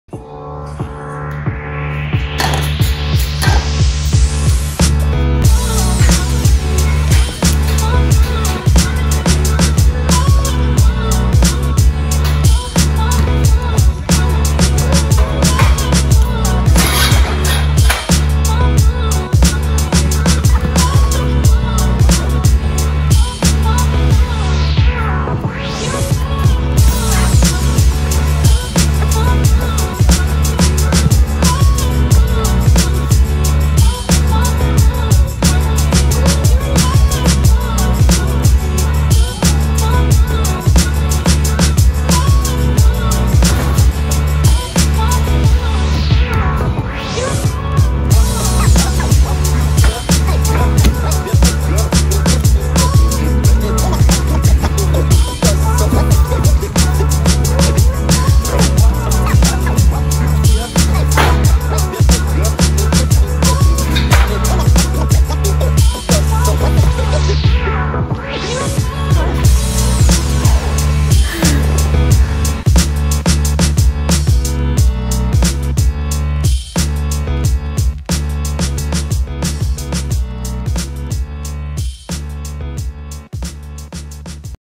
Ninja Warrior Kids Training Video
American Ninja Warrior Junior - style training at Obstacle Ninja Academy in Orlando. Ninja skills on display as this ninja fights through pain to tackle obstacles.

This is my home gym. Come check it out!